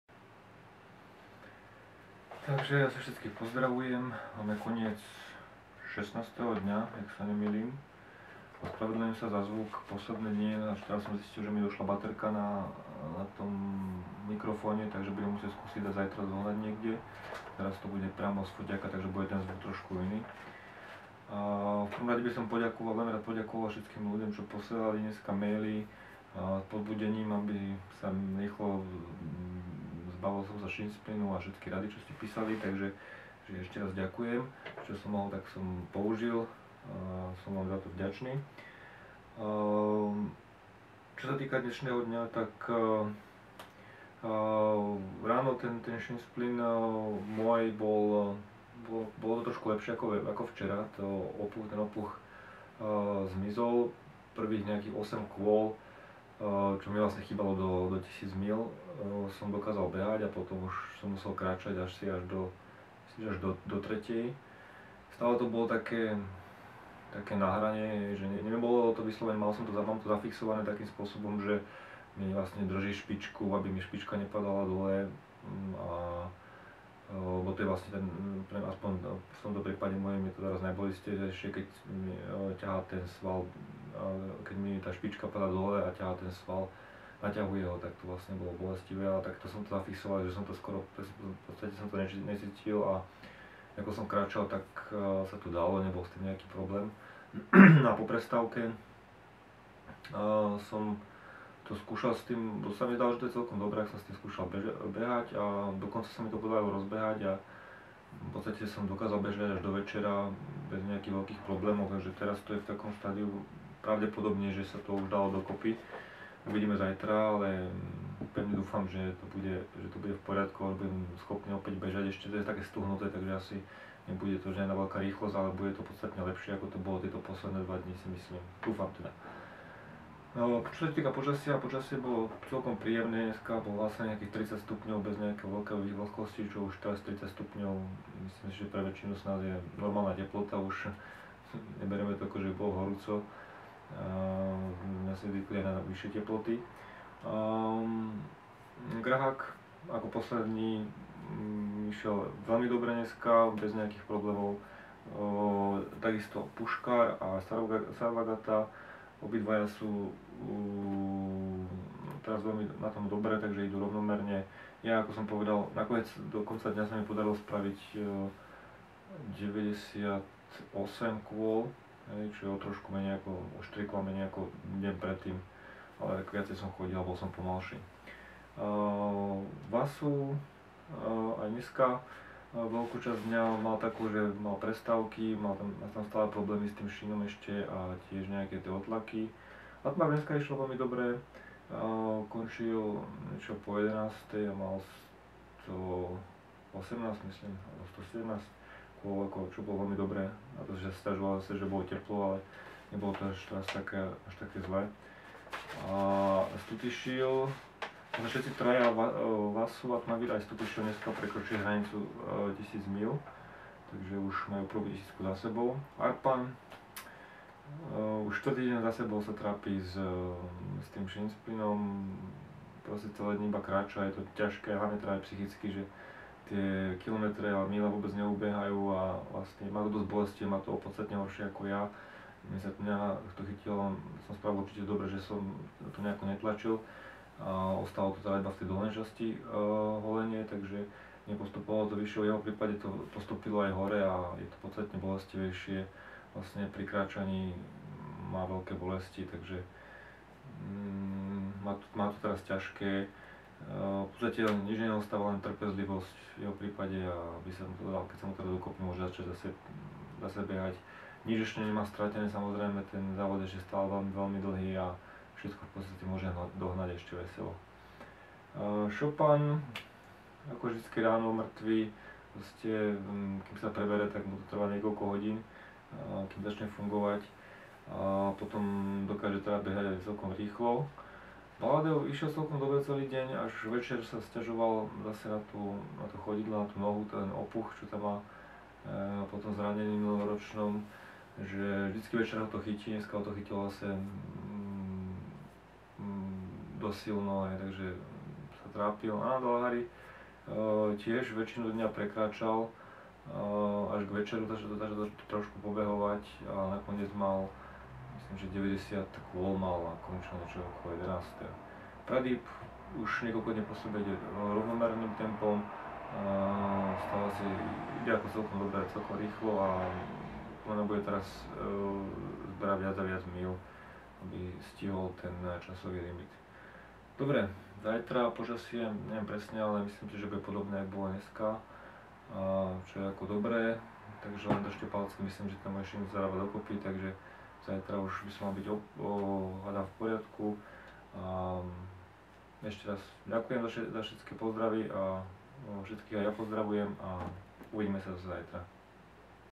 Self-Transcendence 3100 Mile Race 2012 - Deň 16
Najdlhší certifikovaný bežecký závod na svete. Viac informácií nájdete na stránke